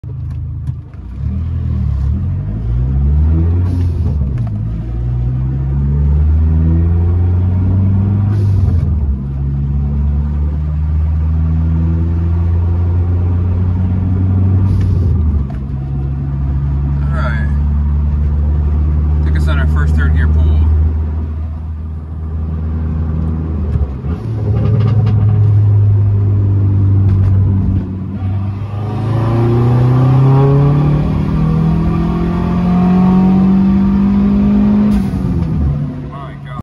2004 Dodge Neon SRT-4 - E85 + BWS - Mid tuning with Needlesstorque.com
Early revisions from my guy, Dave Trojahn. My favorite SRT-4 Tuner in the scene with Needlesstorque. low/mid boost levels. Sorting out rev ranges, everything is coming out fantastic!

NOTE: This AI video is highly edited and visual digital entertainment only. Not real.